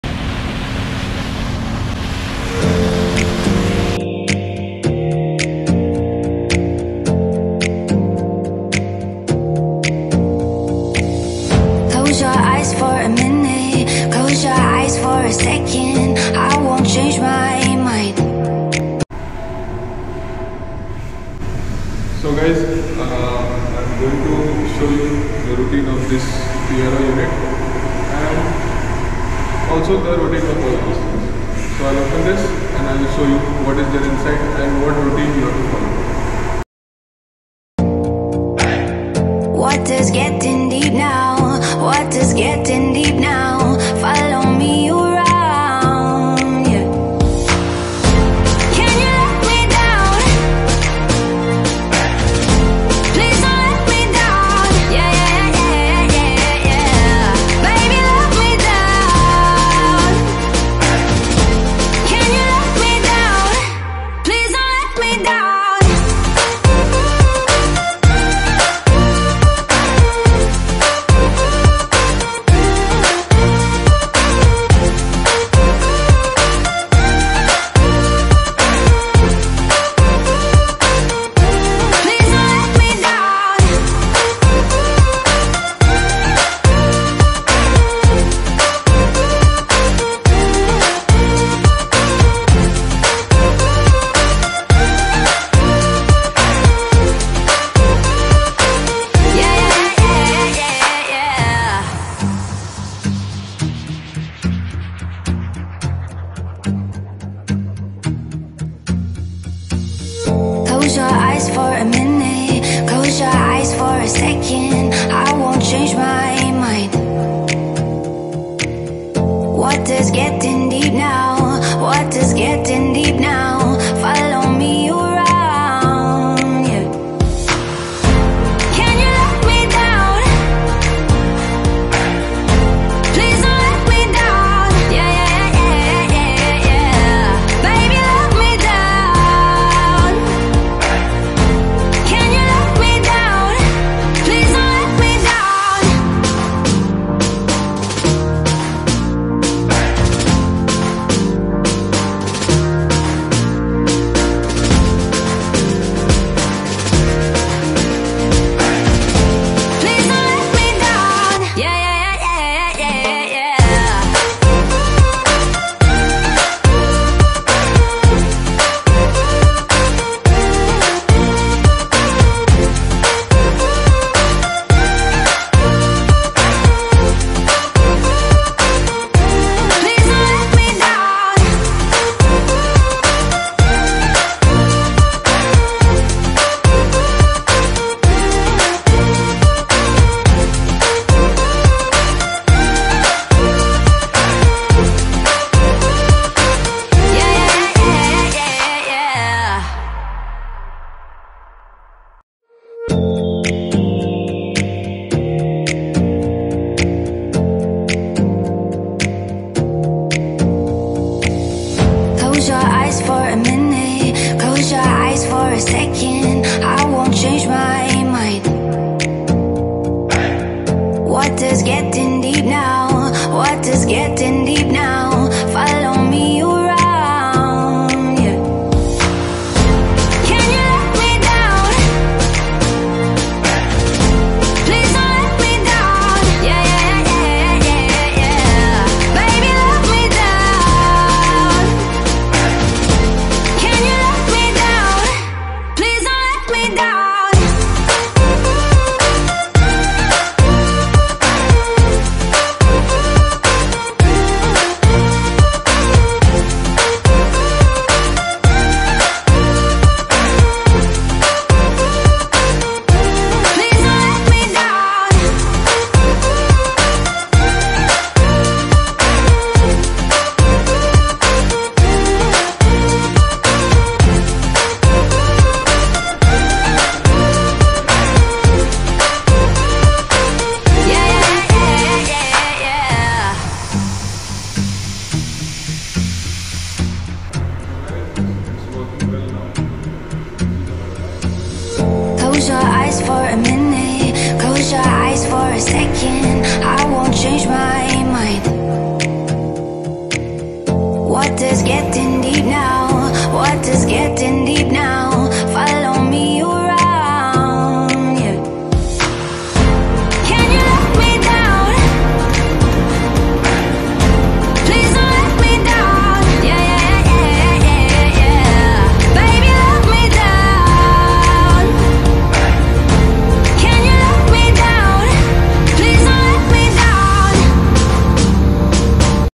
TRO ROUTINE of SUNRUI BalClor BWTS System | Ballast Water Treatment System | Total Residual Oxidants
TRO Basic Routine maintenance of SUNRUI BalClor ballast water treatment system.

This video will help you to know the basic routine required in TRO unit which helps your plant run smoothly.

If you find the video informative, do like it.

Music Credit
Musician: DaFox